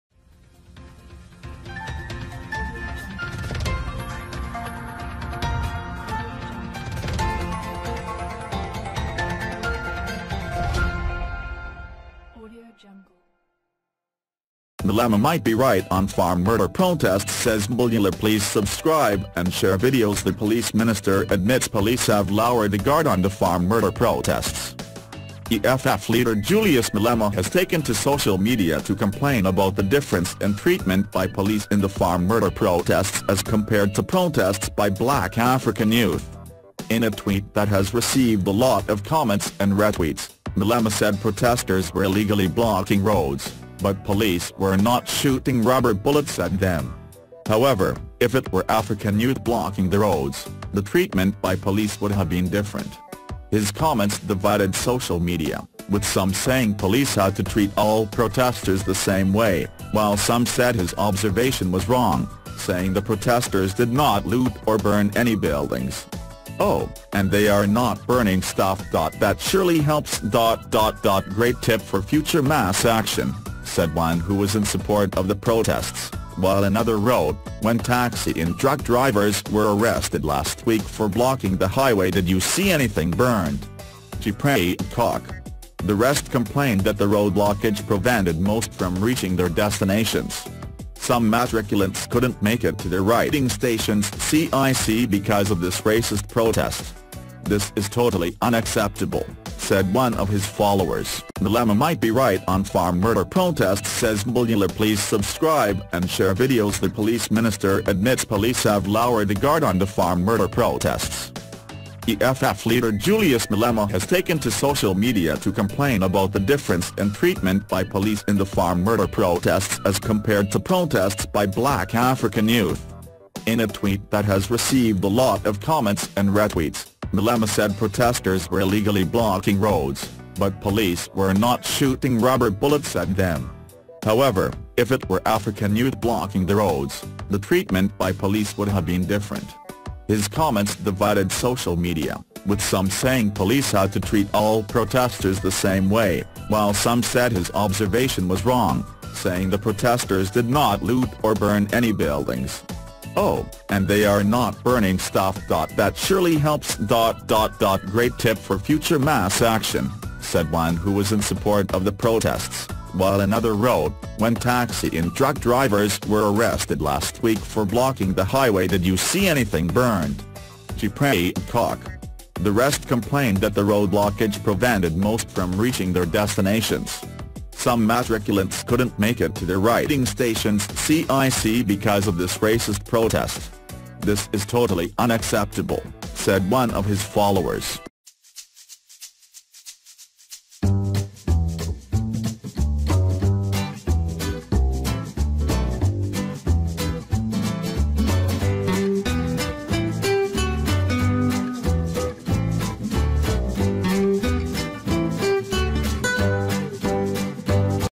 Unbelievable Malema might be right on farm murder protests says Mbalula
Unbelievable Malema might be right on farm murder protests says Mbalula. The police minister admits police have ‘lowered the guard’ on the farm murder protests.

EFF leader Julius Malema has taken to social media to complain about the difference in treatment by police in the farm murder protests as compared to protests by black African youth.

In a tweet that has received a lot of comments and retweets, Malema said protesters were “illegally” blocking roads, but police were not shooting rubber bullets at them. However, if it were African youth blocking the roads, the treatment by police would have been different.
His comments divided social media, with some saying police had to treat all protesters the same way, while some said his observation was wrong, saying the protesters did not loot or burn any buildings.